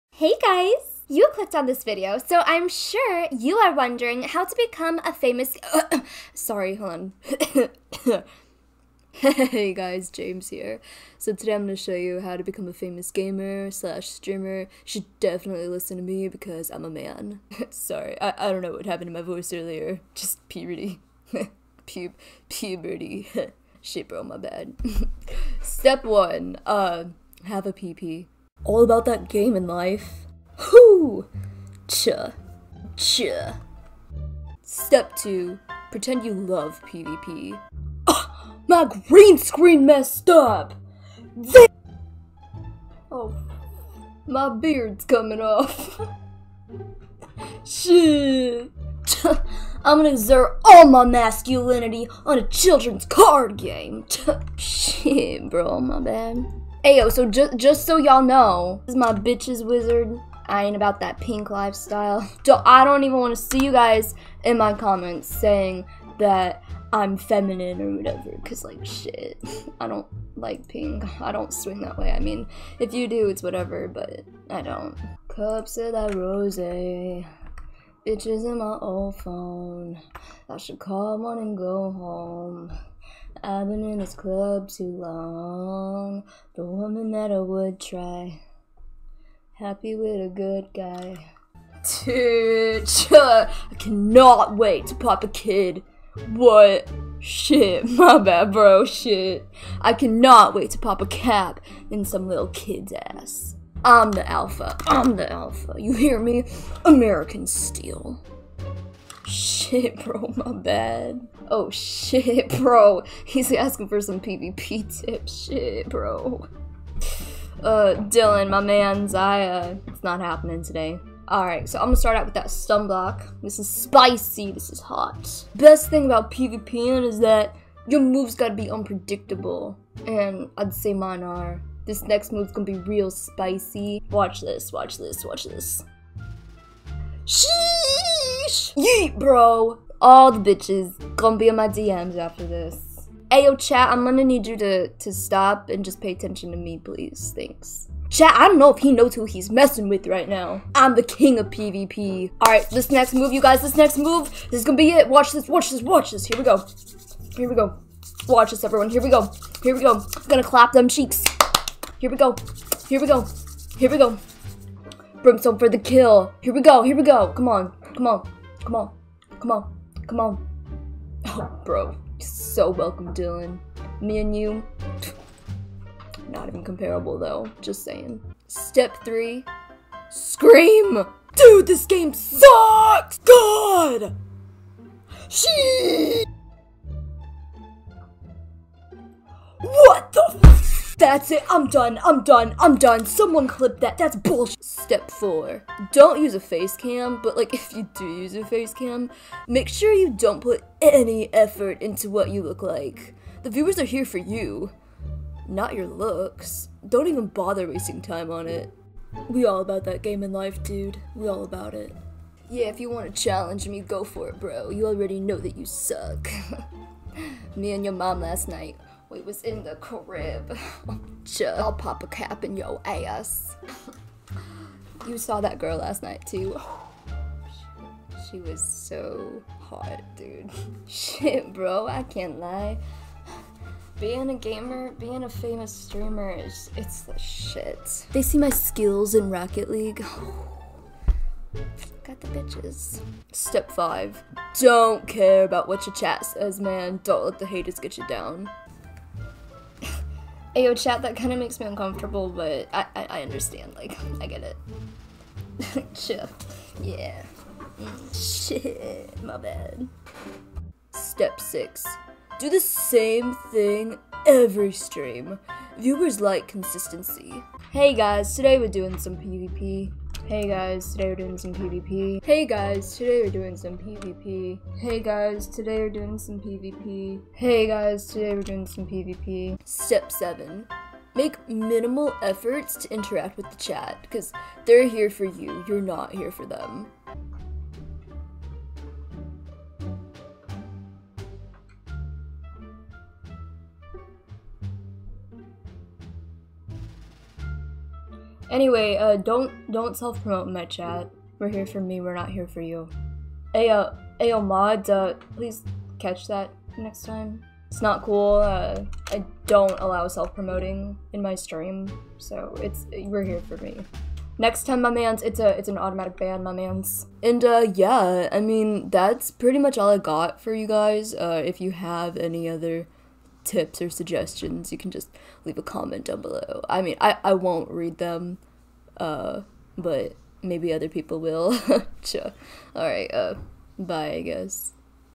HOW TO BE A FAMOUS GAMER/STREAMER!!!
hi all. james here. here's 7 easy tips on how to turn your gaming hobby into your career. hehuh. chuh. feel free to leave comments on some other tips or recommendations for other people. i won't read them but maybe some other people will. idk. hehuh. anyways time to go pvp for the 68th time this week. maybe play some rocket league later or some gta or whatever honestly hehuh. aight bye homies

thanks for all of the support i guess